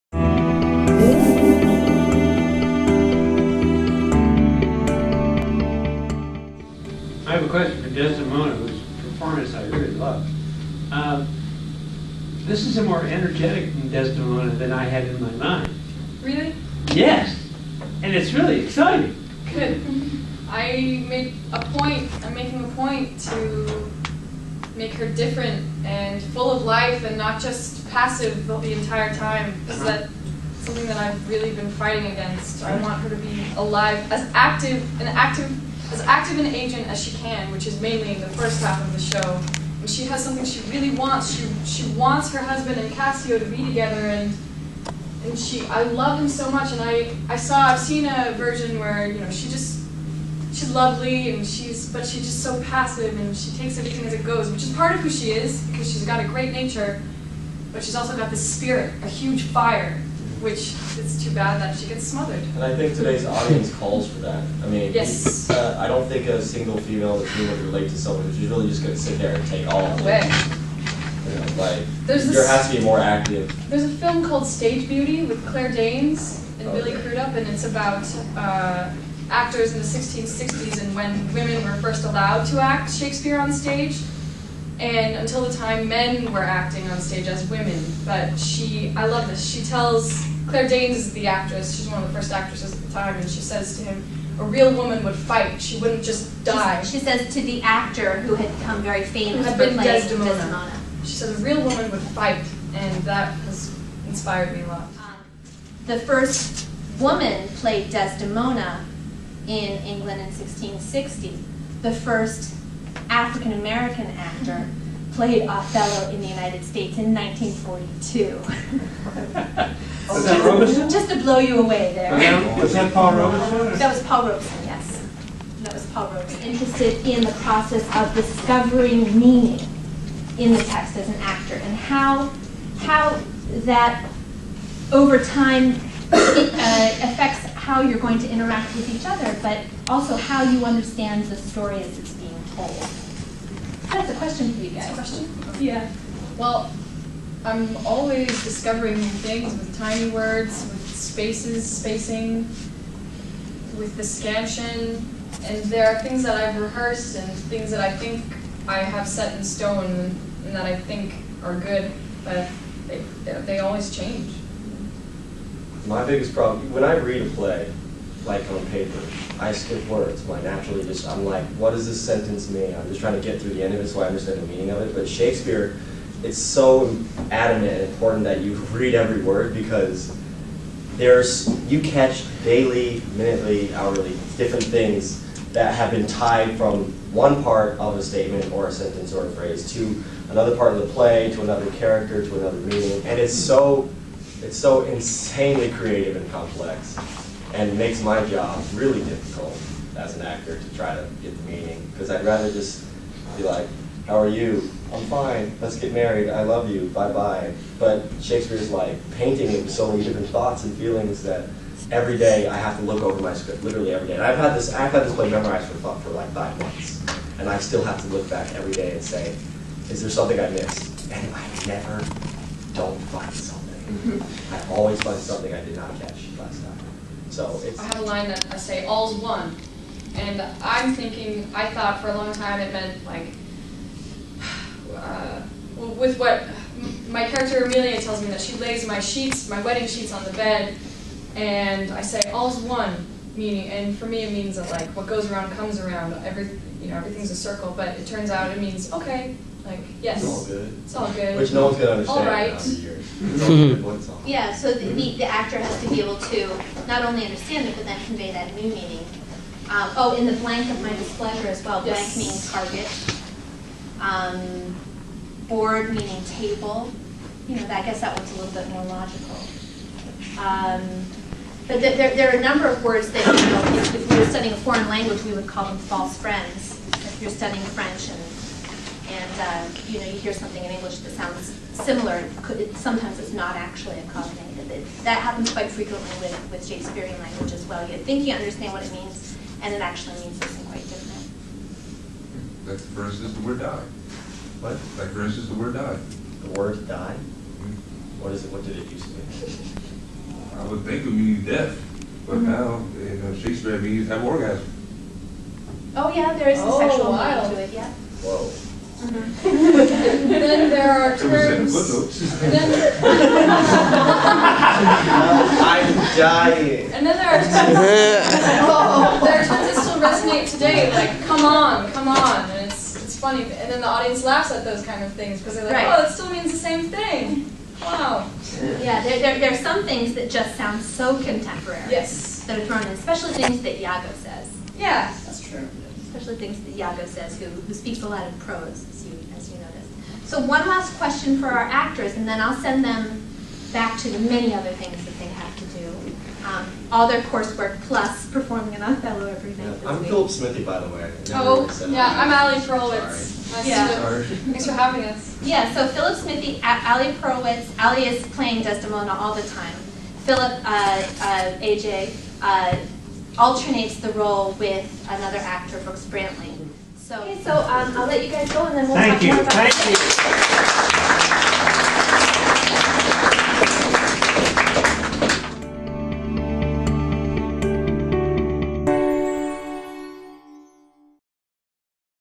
OTHELLO IN THE CLASSROOM, Part II
The OTHELLO Company and Dramaturg Dr. Dassia N. Posner visited several UConn classrooms to perform scenes and  lead discussions.  In this video, Alexandra Perlwitz (Desdemona) and Phillip AJ Smithey (Othello) join Dr. Posner in an English classroom.

Nafe Katter Theatre
University of Connecticut
Directed by Dale AJ Rose

Iago has a plan. At once a love story and a psychological thriller, Othello exposes how one man's envy can wreak havoc in an entire community. A magnificent meditation on trust, jealousy, love and passion, Othello remains strikingly resonate 400 years after it was written, which is why it is one of Shakespeare's most produced plays today.